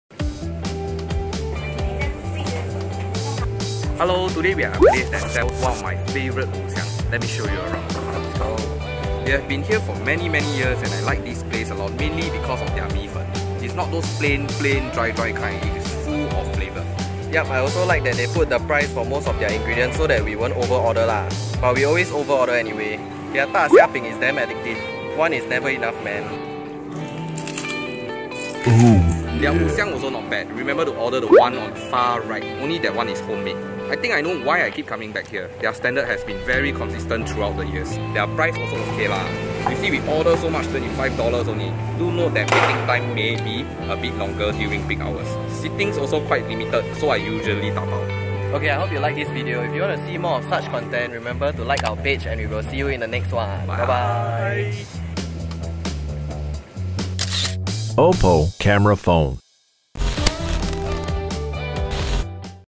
Our favourite 五香! Been eating this for 10 years!
Hidden beside a coffeeshop!
📷Video is filmed with OPPO R15 Phone 📱
Other than all the fried stuff, I think the bee hoon plays a super big part on why i keep coming back. Like comfort food like that. Eat finish 五香 liao can eat dessert which right beside this shop.
Ngoh Hiang
S530682
Opening hours: 10am - 10pm
#01-372
📷 OPPO R15 Phone